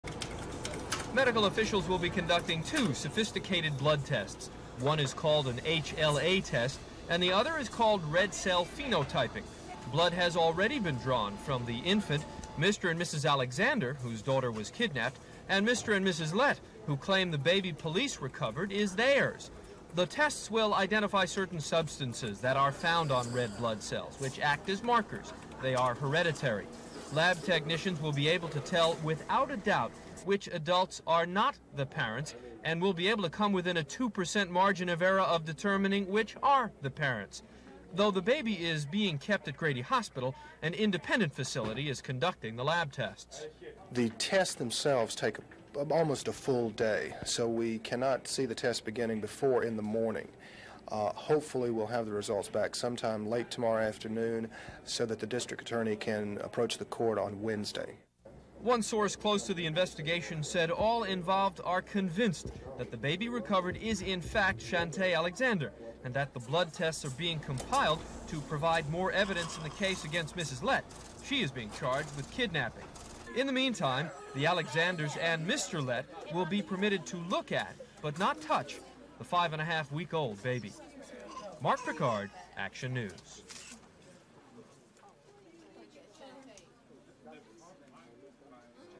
Sandra & Bobby Alexander Await Blood Test to Get Stolen Baby Back (September 14, 1981)
Natural sound and voice over footage of: medical laboratory, medical examiners conducting test to determine the parents of a baby being kept at Grady Hospital, Hospital official speaking to reporter about timeline, baby victim of alleged kidnapping.
Reporter: Pickard, Marc